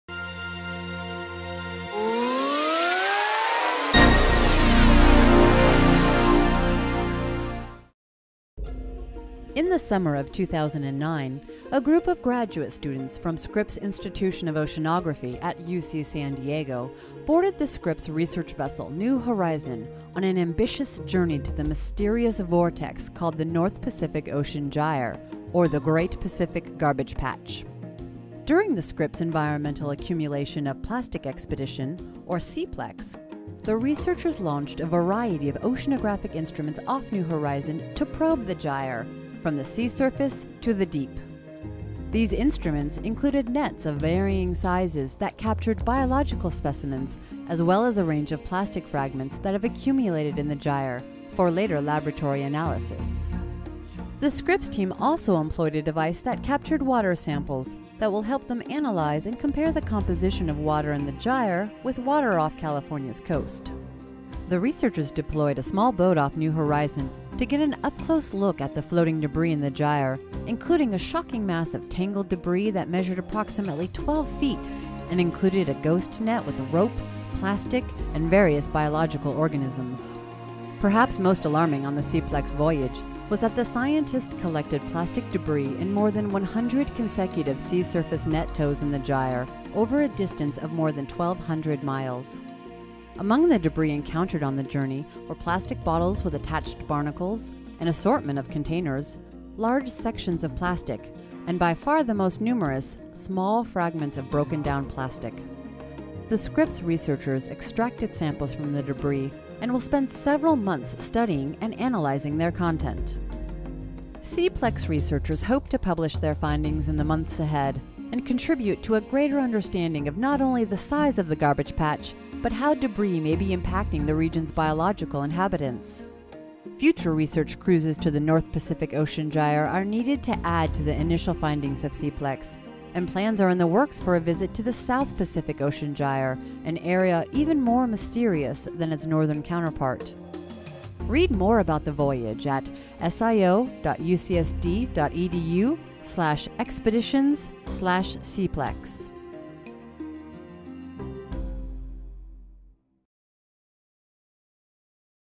(Narrated) Footage from SEAPLEX Voyage to the Great Pacific Garbage Patch
From August 2-21, a group of doctoral students and research volunteers from Scripps Institution of Oceanography at UC San Diego embarked on an expedition aboard the Scripps research vessel New Horizon exploring the problem of plastic in the North Pacific Gyre.

This video was produced for Scripps Institution of Oceanography's FREE, award-winning, explorations e-magazine.